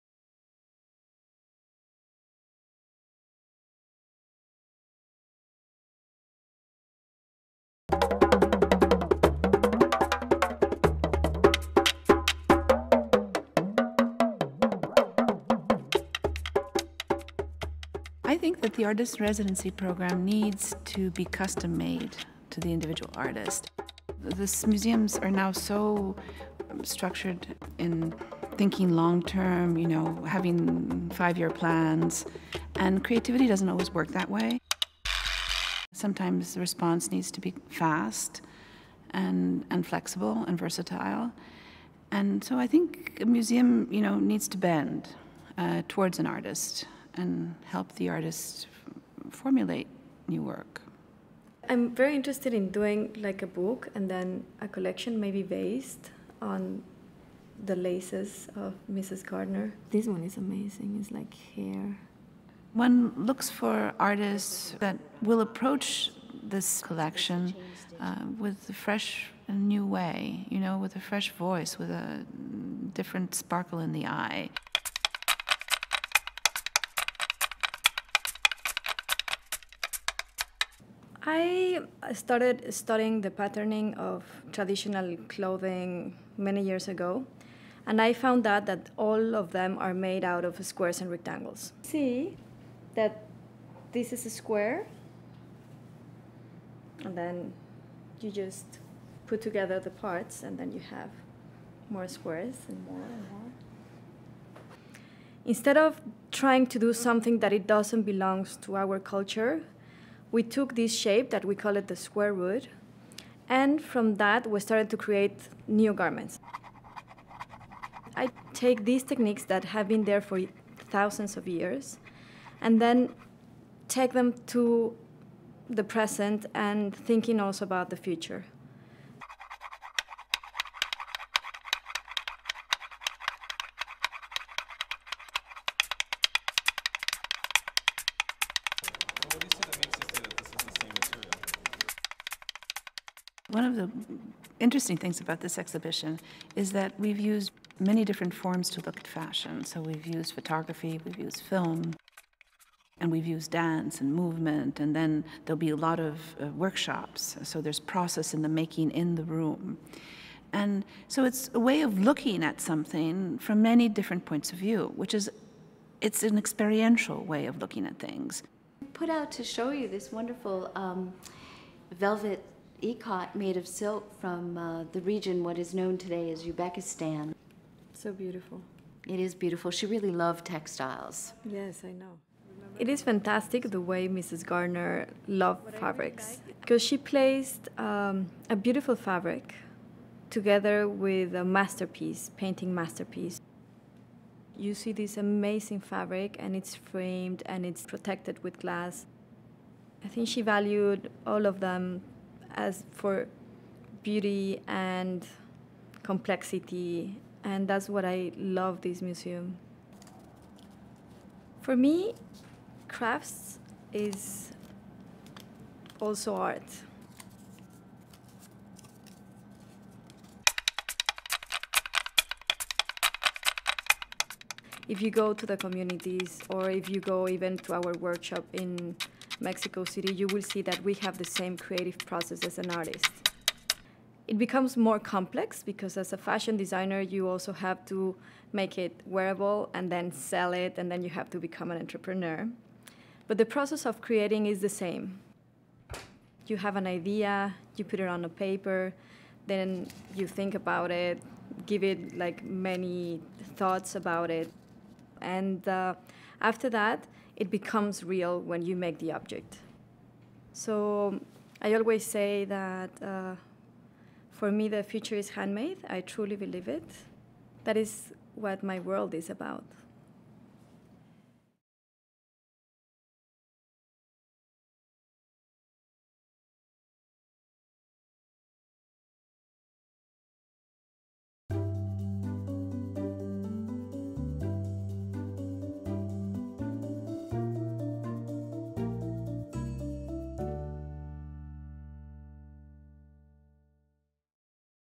Carla Fernández: The Barefoot Designer
Carla Fernández: The Barefoot Designer: A Passion for Radical Design and Community explored the traditions and techniques of indigenous Mexican artisans and how they can be applied to modern fashion and styles.

Carla Fernández has gained international recognition for her extraordinary approach to documenting and preserving the rich textile heritage of Mexico's indigenous communities by transforming it into beautiful contemporary clothing, and proving tradition is anything but static.

This first-ever fashion exhibition at the Gardner Museum explored the development of a new language in visual design that Fernández has built over two decades. She used a method called "the Square Root" based on the Mexican tradition of making clothing from squares and rectangles. This process emphasizes forms of fabric and delicate, thoughtful construction based on whole fabric, as opposed to cutting in curves and molding to the body.

Learn more about contemporary art at the Gardner Museum on our